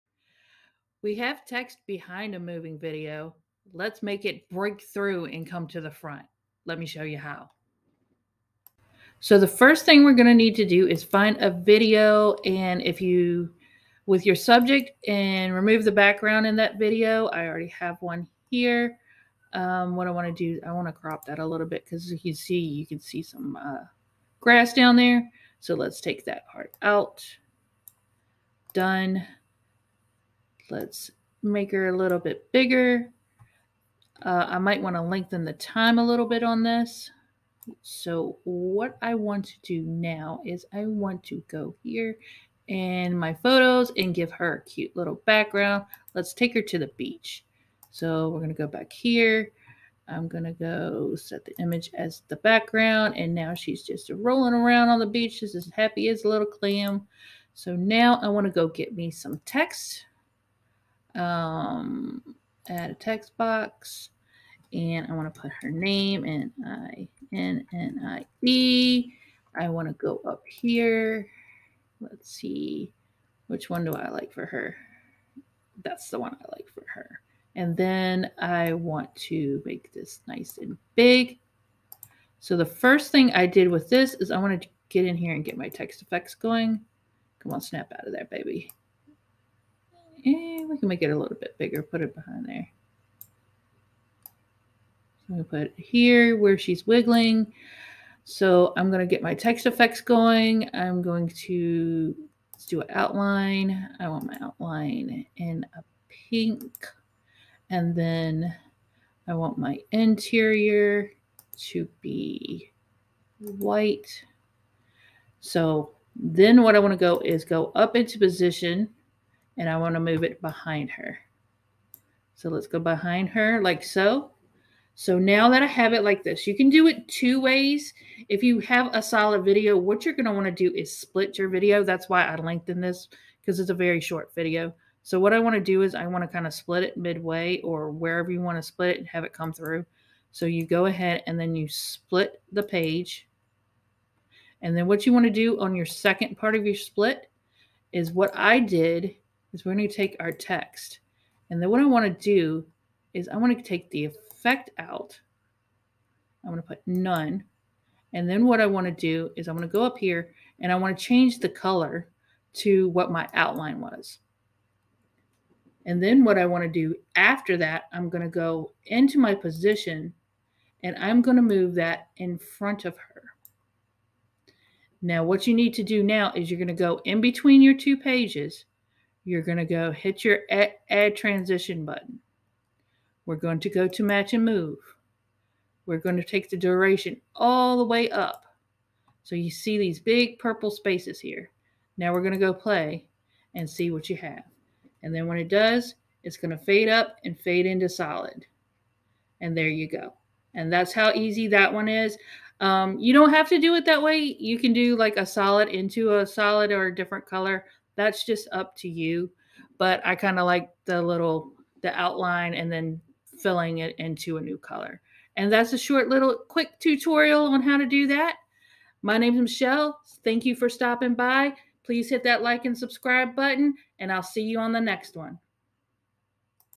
How to Create the Break through Text Effect
Have ever wanted to give your text a unique flair?  This Canva tutorial will show you how to create the illusion of your word or phrases going through your image or video. This graphic design was created in Canva with a free account and how easy it is to do.

All videos are 100% made in Canva